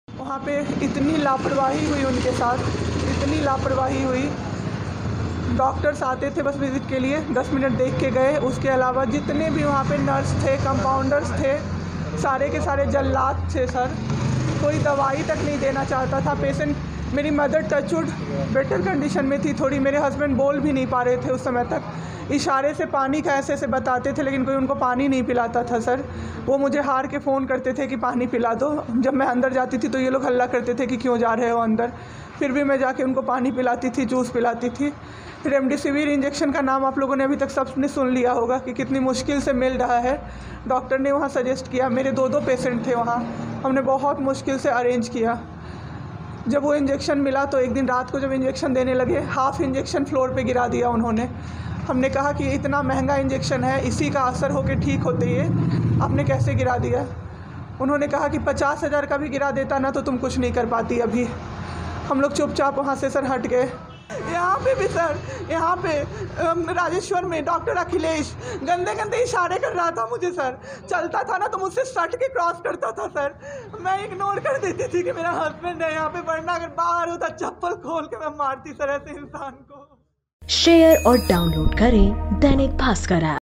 महिला की दिल दहलाने वाली दास्तान
पति भर्ती थे, नहीं तो डॉक्टर को चप्पल से मारती: महिला की दिल दहलाने वाली दास्तान; अस्पताल वाले ऑक्सीजन जानबूझकर बंद कर देते ताकि हम महंगी ऑक्सीजन खरीदें, फिर भी जान नहीं बचा सके